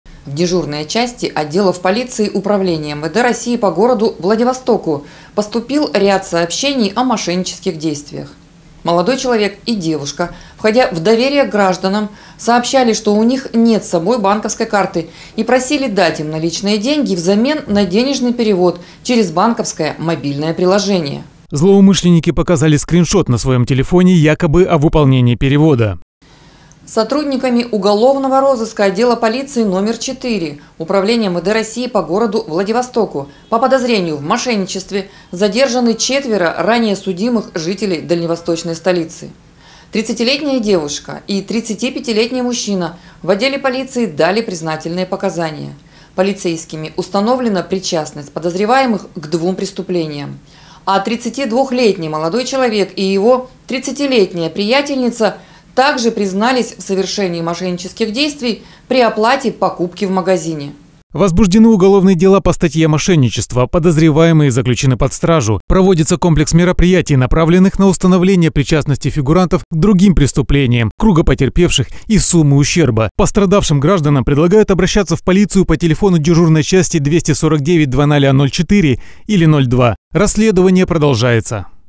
Во Владивостоке полицейские задержали подозреваемых в мошенничестве.
В дежурные части отделов полиции УМВД России по городу Владивостоку поступил ряд сообщений о мошеннических действиях со стороны неизвестных молодых людей. Молодой человек и девушка, входя в доверие к гражданам, сообщали, что у них нет с собой банковской карты и просили дать им наличные деньги взамен на денежный перевод через банковское мобильное  приложение.